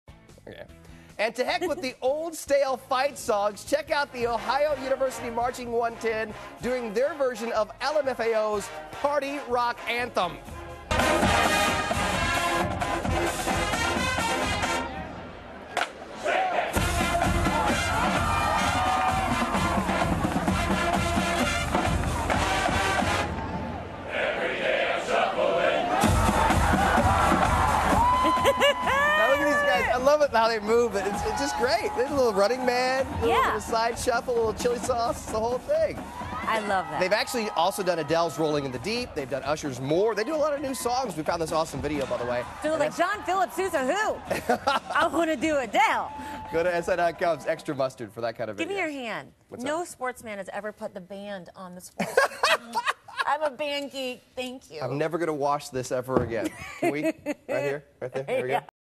Why did Robin Meade kiss Carlos Diaz's hand? Blame Ohio
Did you know that Robin Meade is a proud 'band geek?' Flute, marching bells, piccolo, oboe, french horn... she played them all AND she was a majorette. Which is why she was so pleased to see this clip:Yes... as a proud halftime performer... Robin was keen to give props to a sports guy who shined a spotlight on the band!